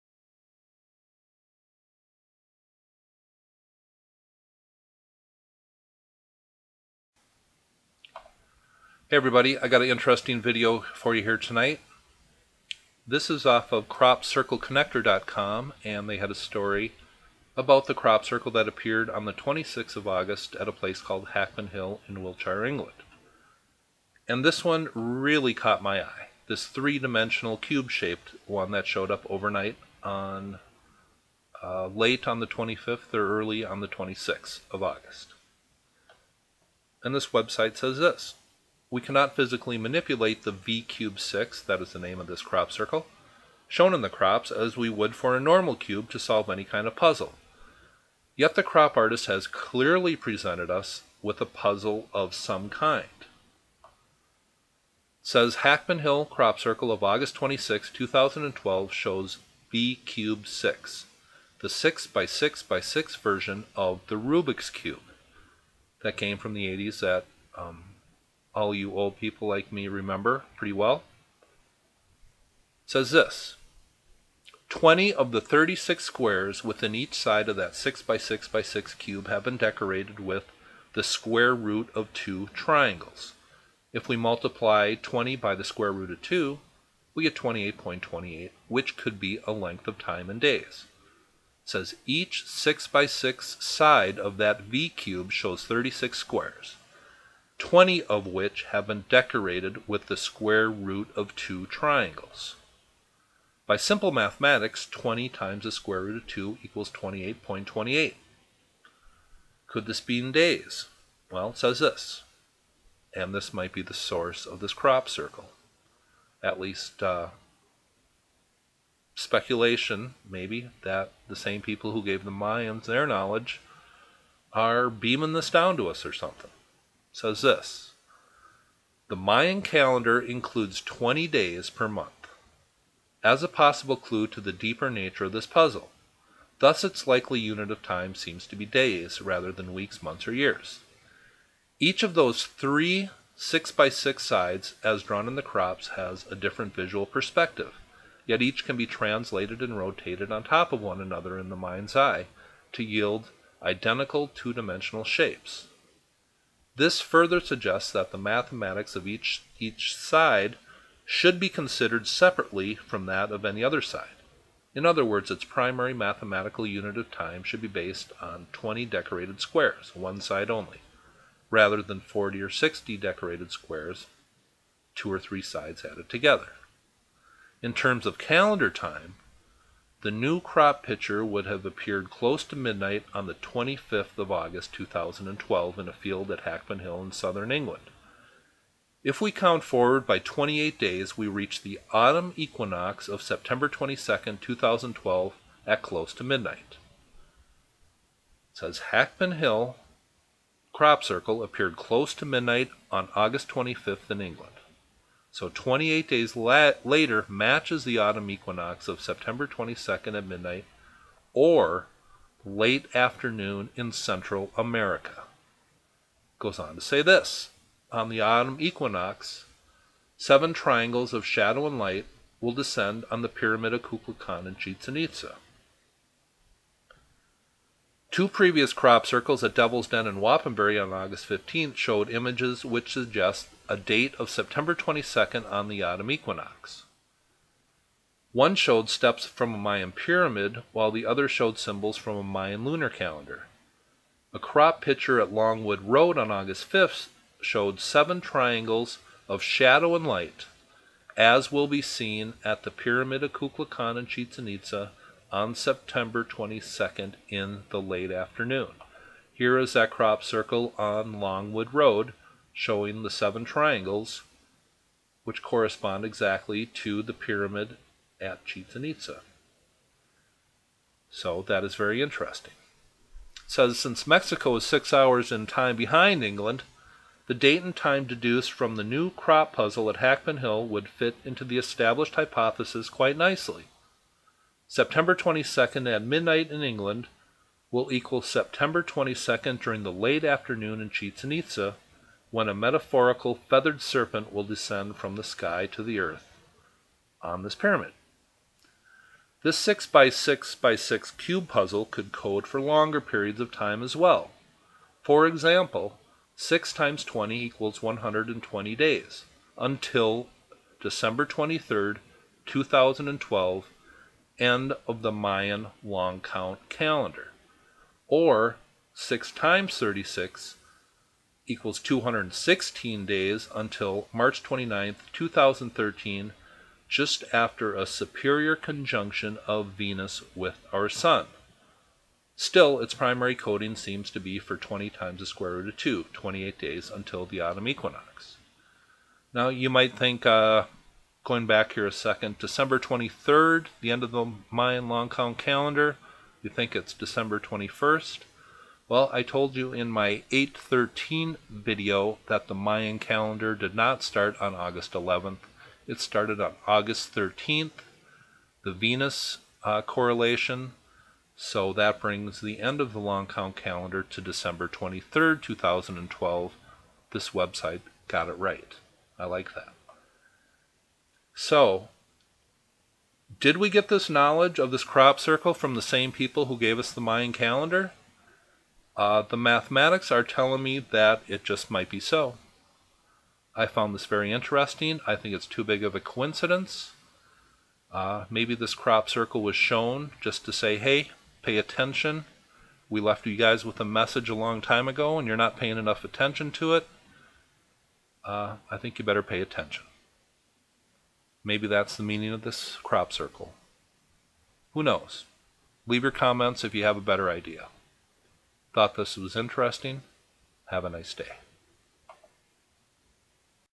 Mayan Math Of Hackpen Hill Crop Circle
The amazing crop circle that showed up on the 26th of August at Hackpen Hill near Wiltshire, England, has Mayan Calendar mathematics incorporated into it.  A message from those who gave the Mayans their knowledge?